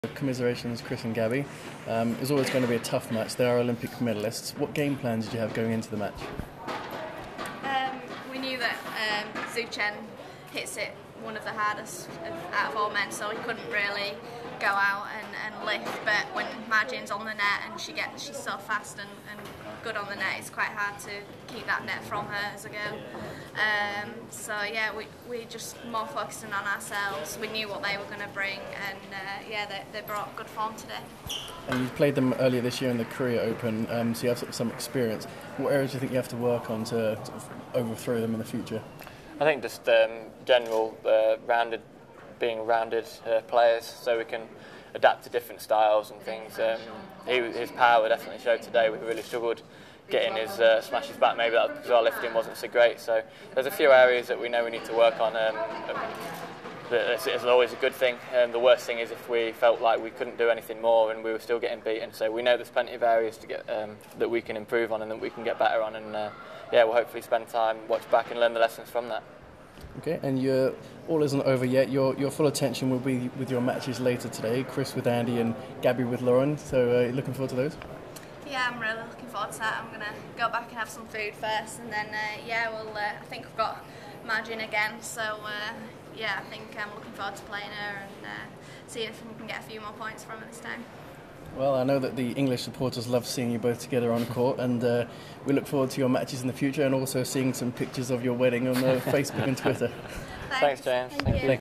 Chris and Gabby post match interview
Interview with Chris Adcock and Gabby White after their defeat against China's Olympic silver medalists Xu Chen and Ma Jin today at the Yonex All England Open Badminton Championships